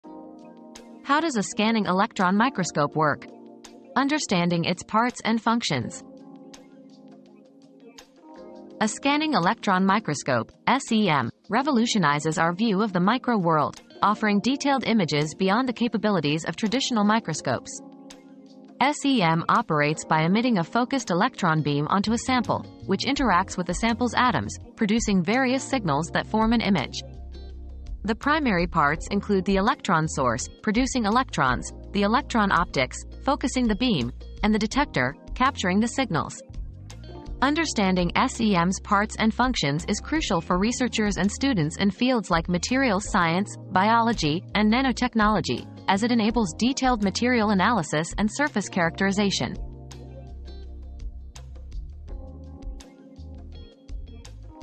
How Does a Scanning Electron Microscope Work? Understanding Its Parts and Functions
Unlocking the Micro-World: How a Scanning Electron Microscope Works • Unleashing the Micro-World • Discover the inner workings of a Scanning Electron Microscope (SEM) and how it reveals the hidden secrets of the micro-world. Explore its primary parts and functions, and learn how SEM empowers researchers and students in materials science, biology, and nanotechnology for precise analysis and surface characterization.